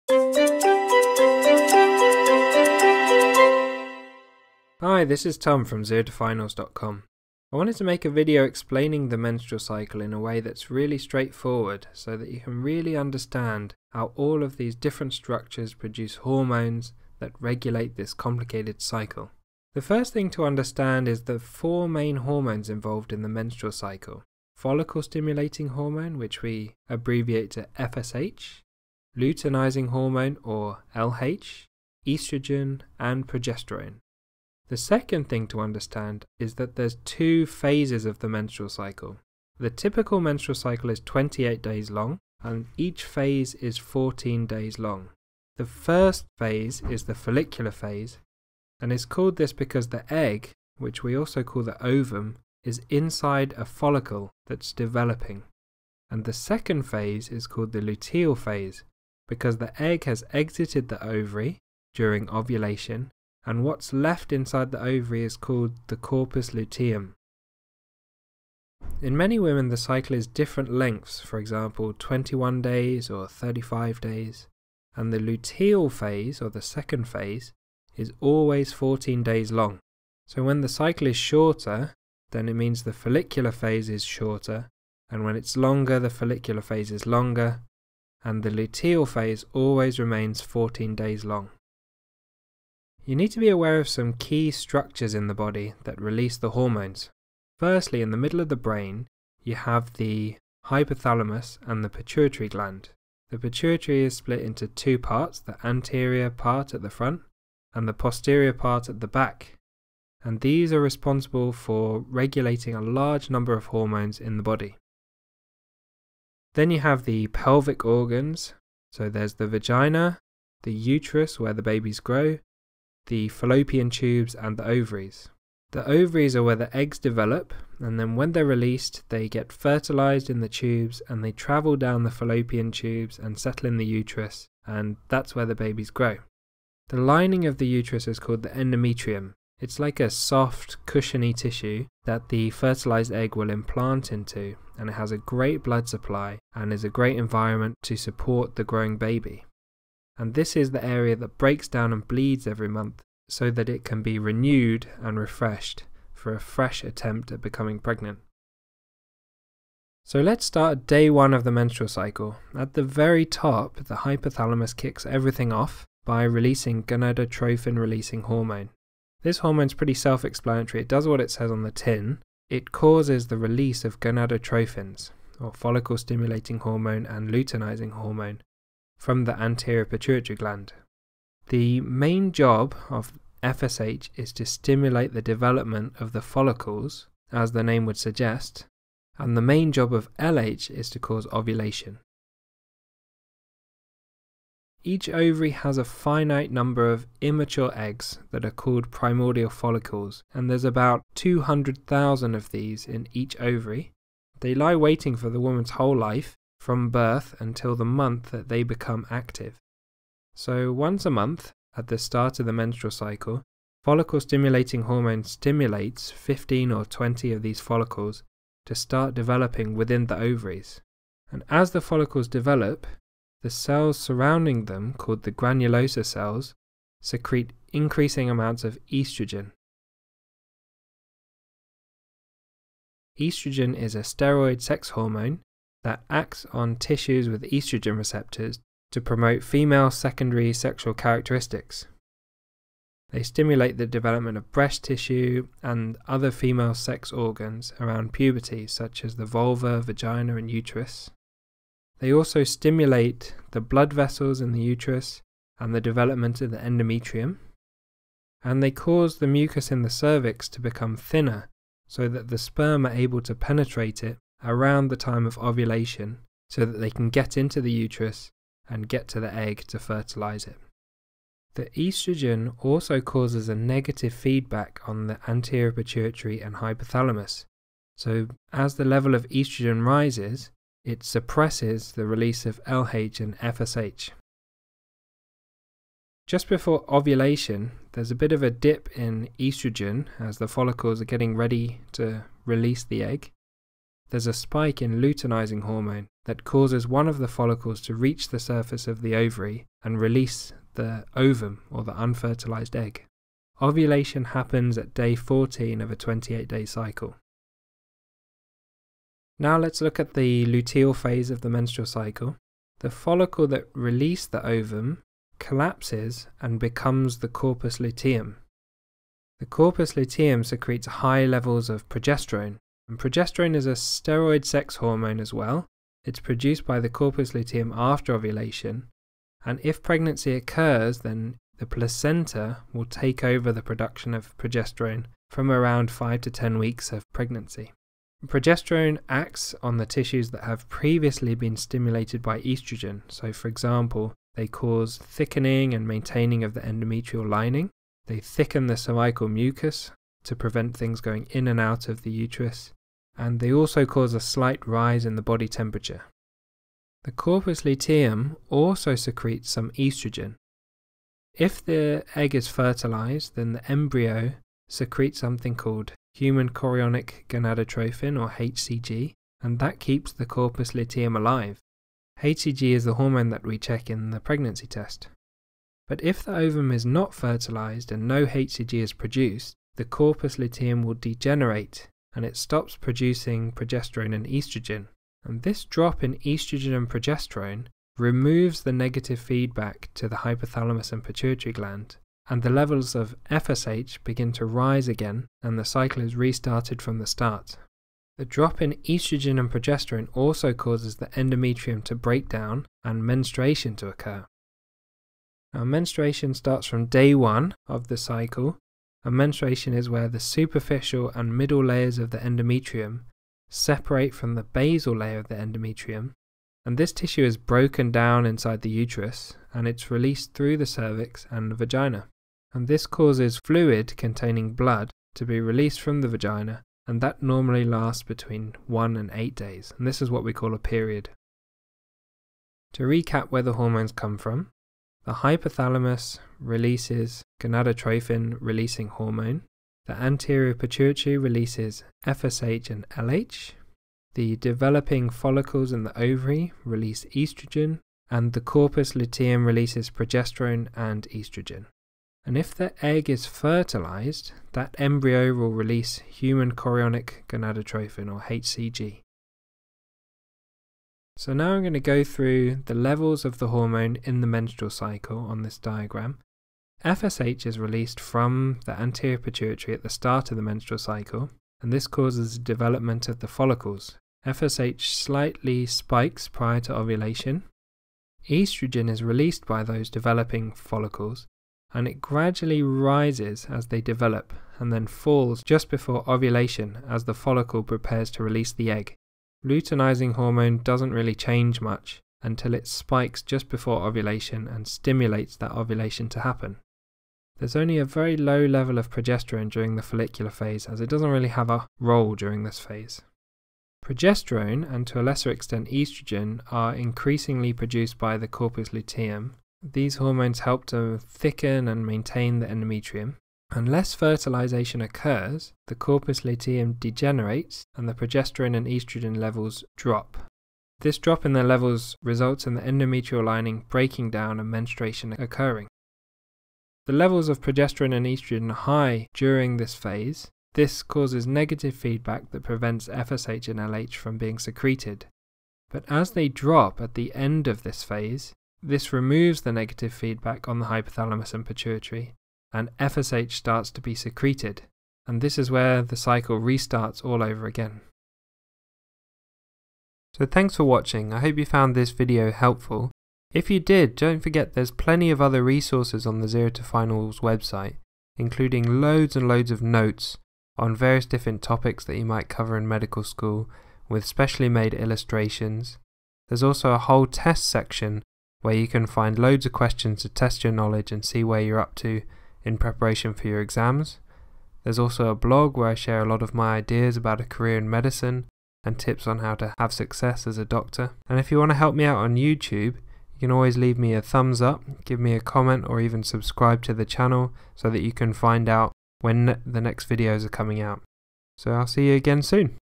Understanding the Menstrual Cycle
This video goes through the hormonal and physiological changes that occur during the menstrual cycle.

*CORRECTION* FSH does not start the development of the follicles. The follicles are constantly developing from primordial follicles to primary follicles to secondary follicles. FSH stimulates further development of a number of these into mature follicles at the start of the menstrual cycle (rather than initiating the development as the video suggests).